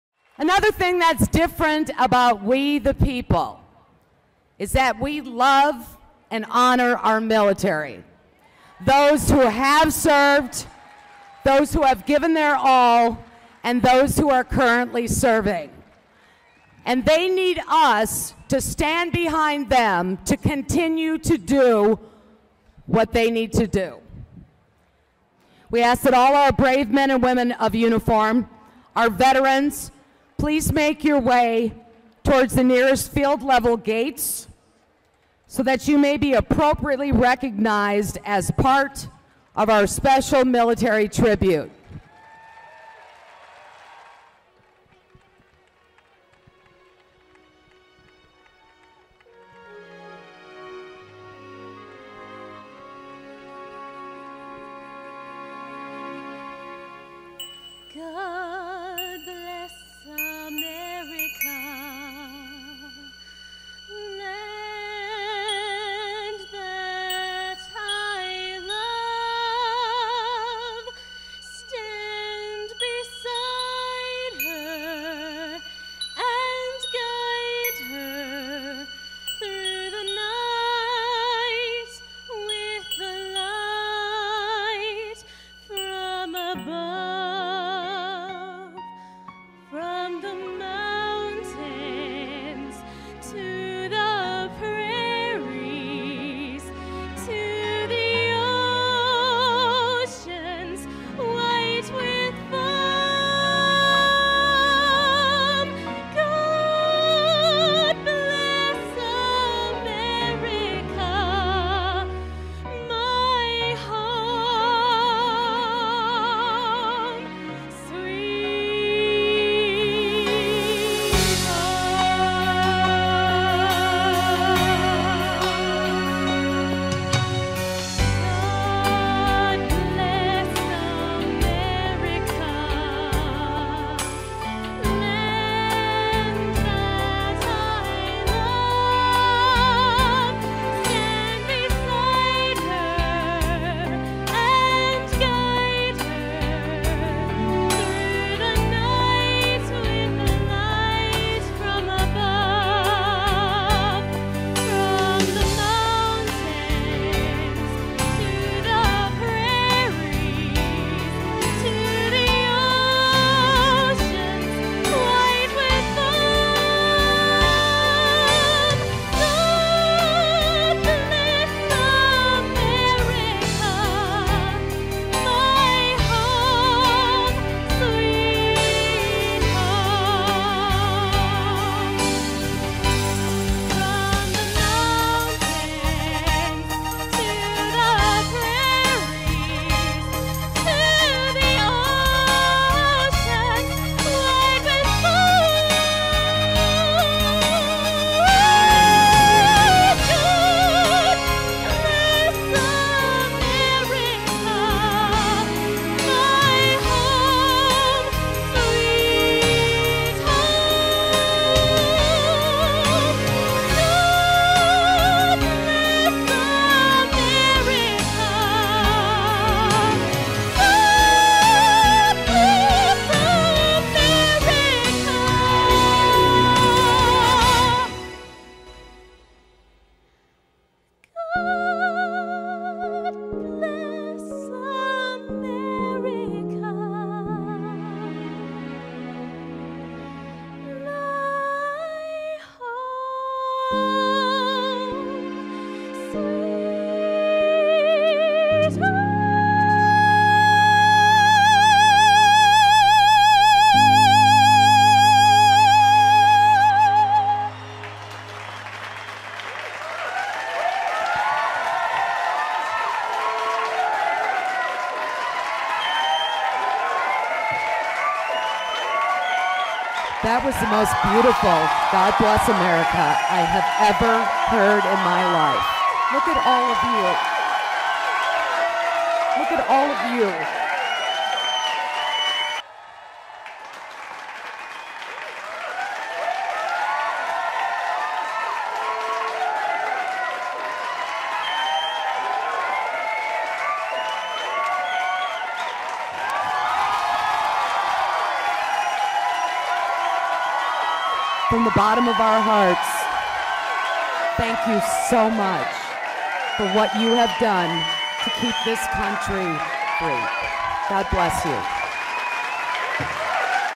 Military Tribute - Part 1
POLITICAL CHIPS TAX DAY TEA PARTY 4/15/2010 AT COMMUNITY AMERICA BALLPARK IN KCK. PART 1 MILITARY TRIBUTE.
DARLA JAYE 980KMBZ RADIO, KORTNEY PYLE, VETERAN HEROS.

THANK YOU FOR YOUR SACRIFICE AND SERVICE!